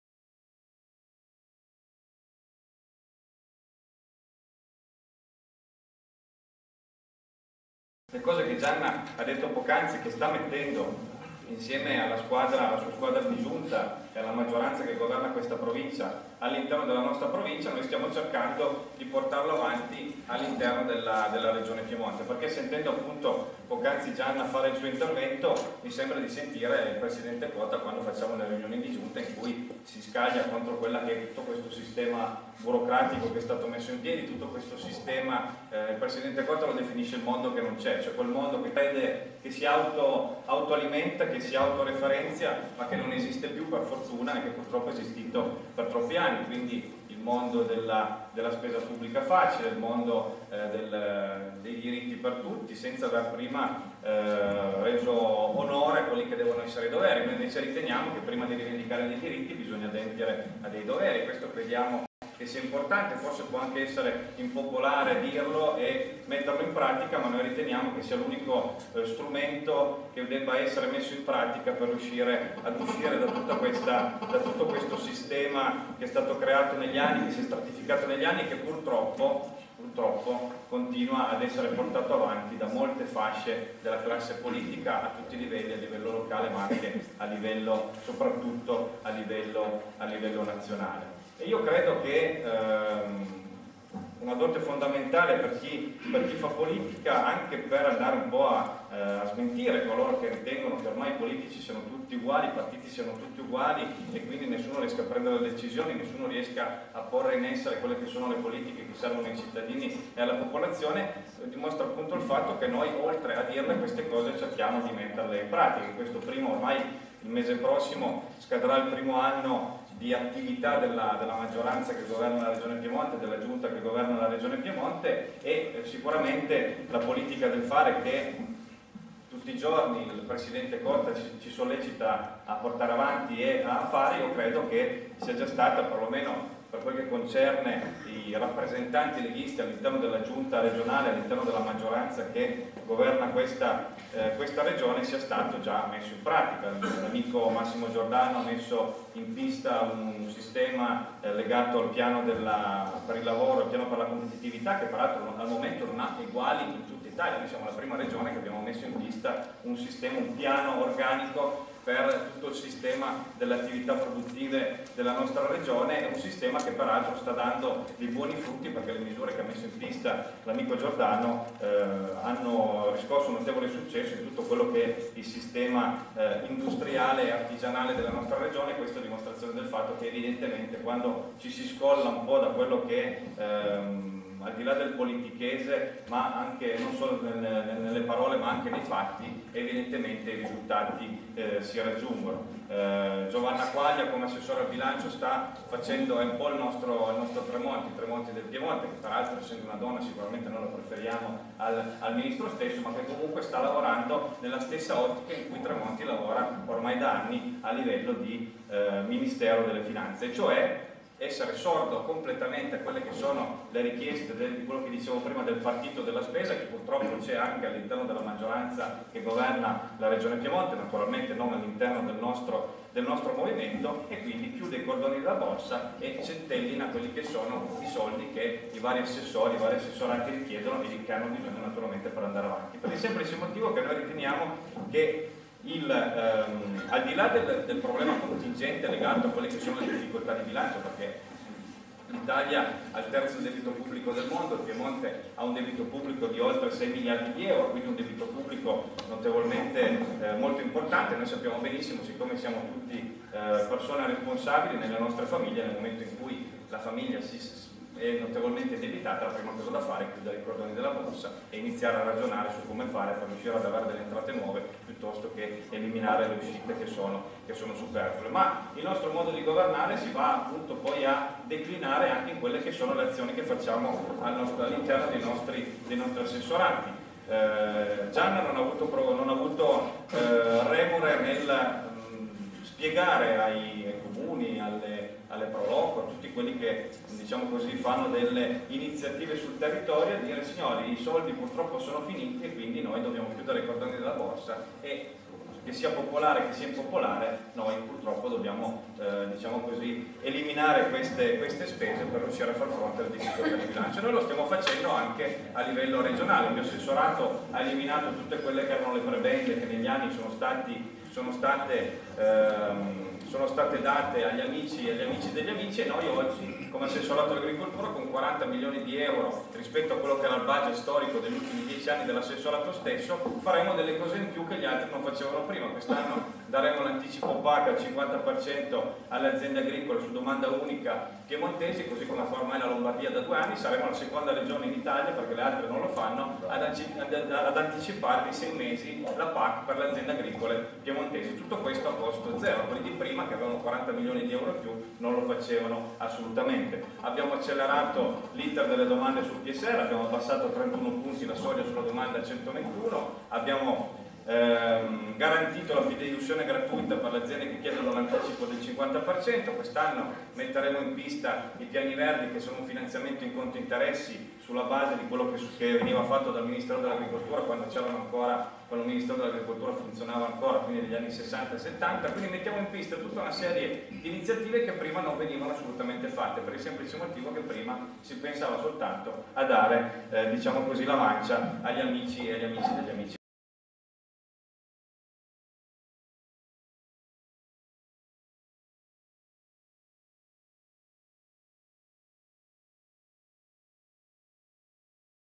Claudio Sacchetto alla Scuola Politica a Fossano (Lega Nord Provincia di Cuneo)
Intervento di Claudio Sacchetto (Assessore Regionale all'Agricoltura) alla Scuola Politica Lega Nord della Provincia di Cuneo a Fossano dell'11 marzo 2011